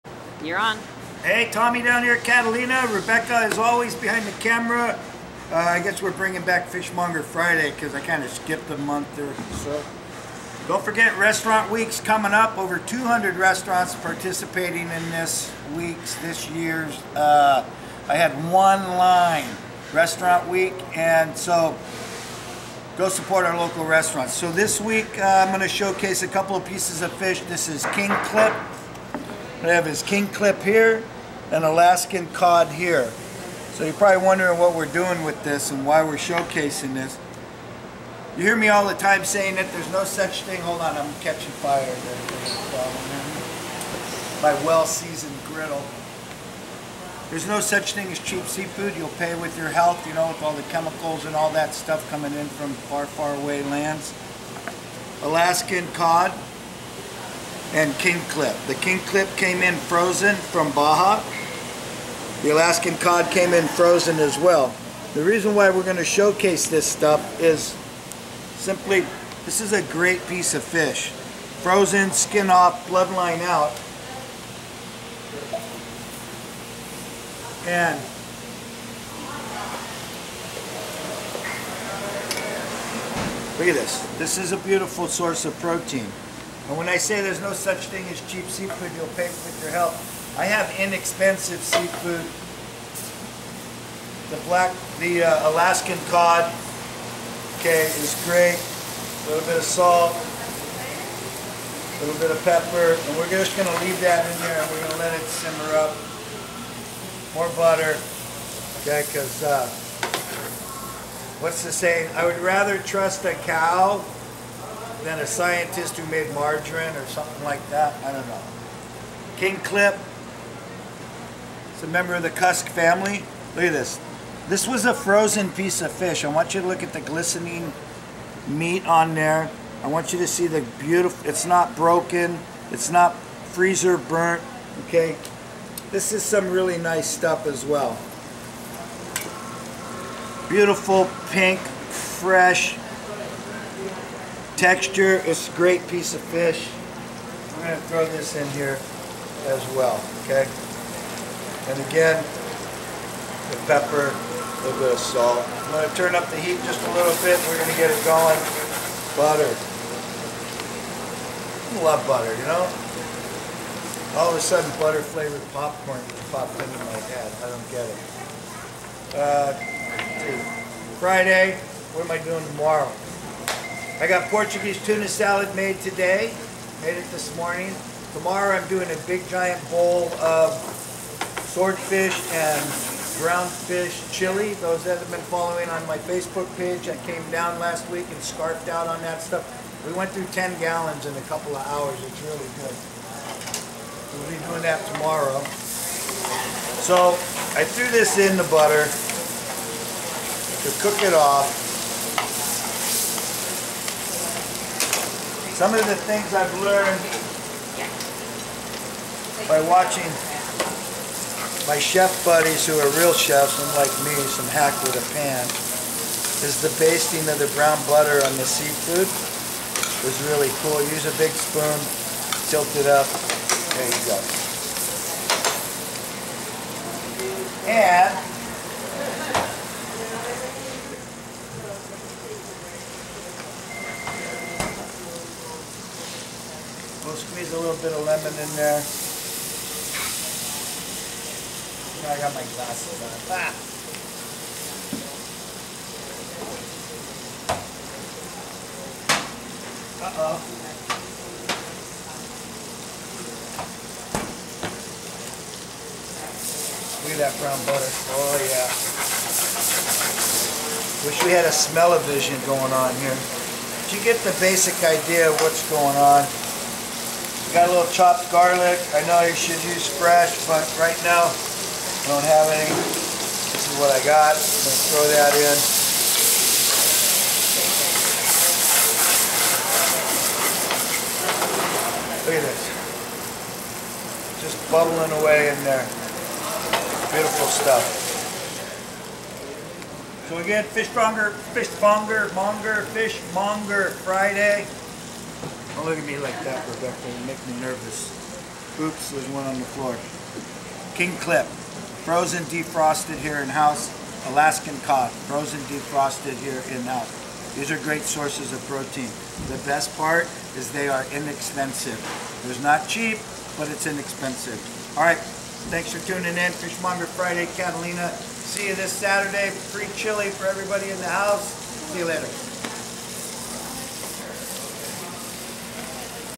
FISHMONGER FRIDAY: Whats in house today. KING KLIP and ALASKAN COD FISH
A simple channel showing easy ways to cook seafood while explaining things like ways of harvesting and how to buy seafood, great little channel